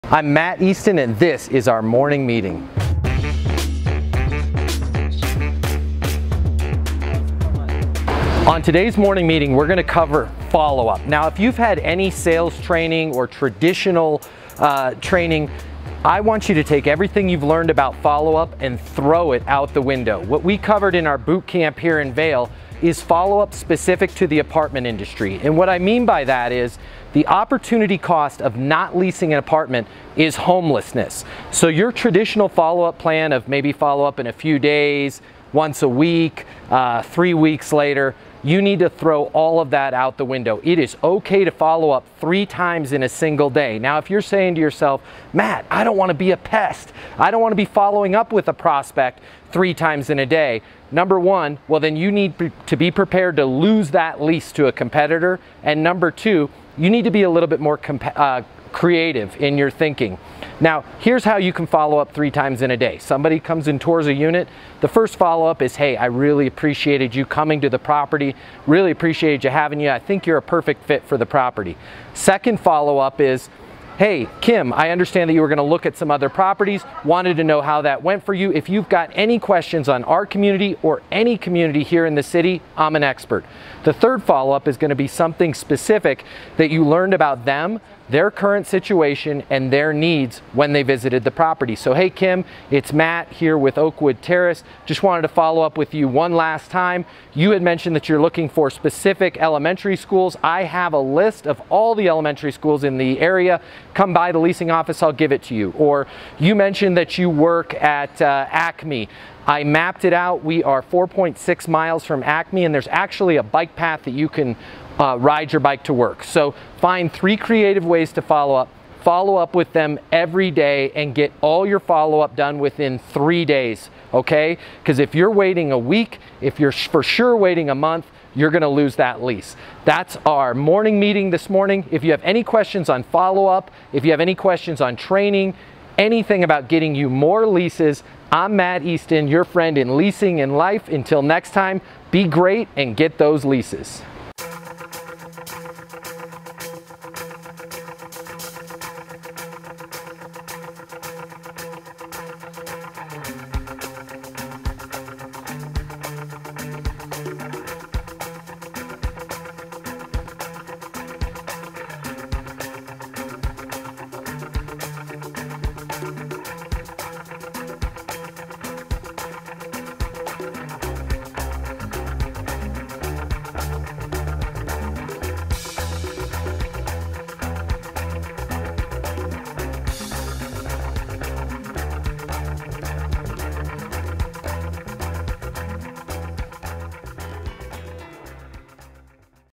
How Often Do I Follow Up?
How often do I follow up? If you have asked yourself this question about follow up calls, then today's Matt Easton Morning Meeting is for you!

Follow up calls are massively important to your success. If you want to close the sale and sign the lease you need to make more follow up calls.

In this video, I am going to show you a simple strategy that will allow you to easily make 3 follow calls in a single day without seeming like a pest or a high-pressure leasing agent.

Following up is critical, watch this video and learn how to follow up like a PRO!